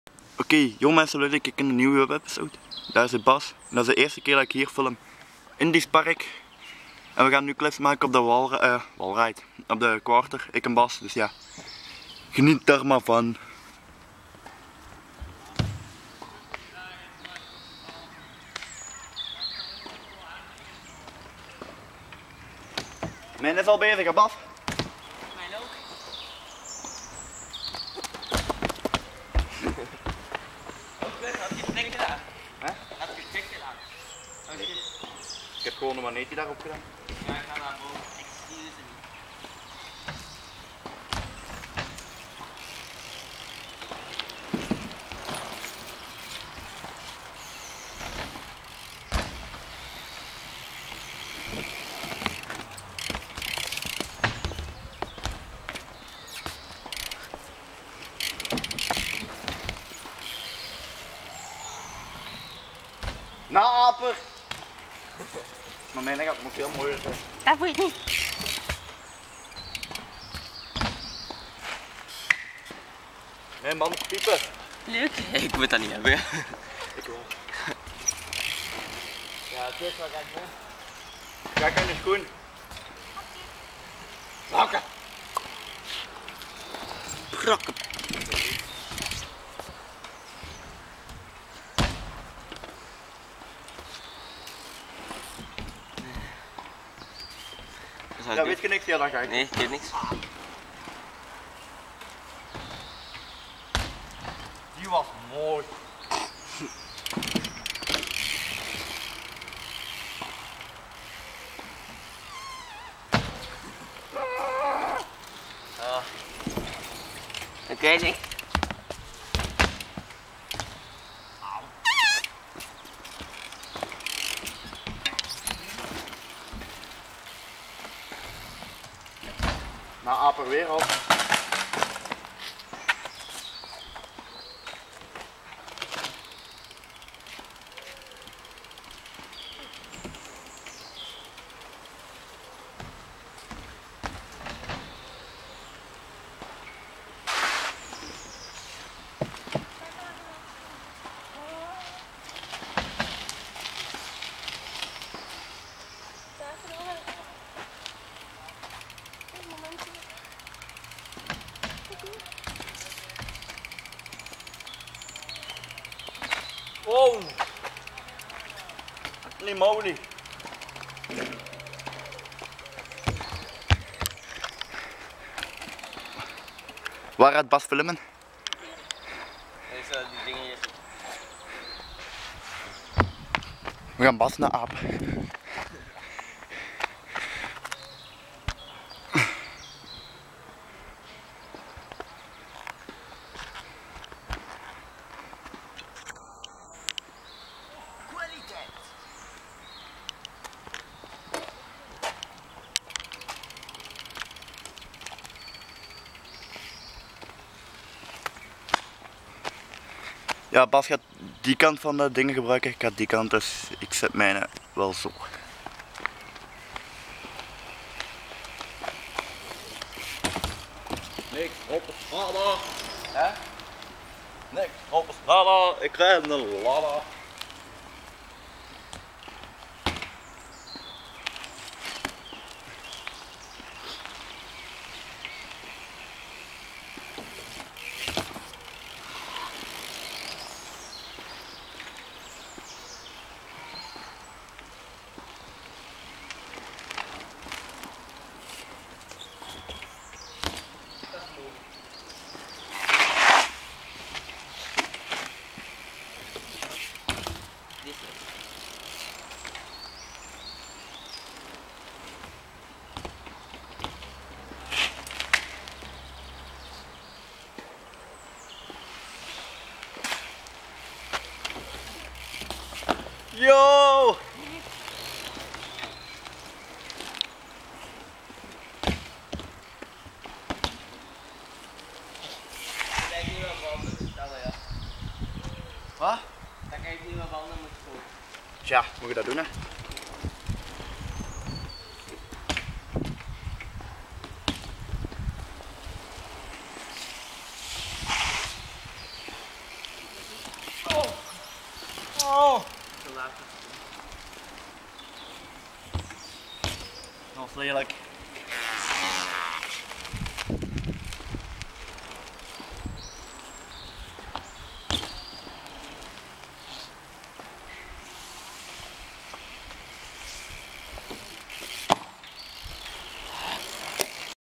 webisode 14: first time filming here at diest skate park
in deze video ben ik samen met bas op diest skate park oftewel warande skate park genoemd ik hoop dat je deze video leuk vond als je deze video leuk vond laat dan een like achter en abonneer op mijn kanaal en check ook bas zijn kanaal uit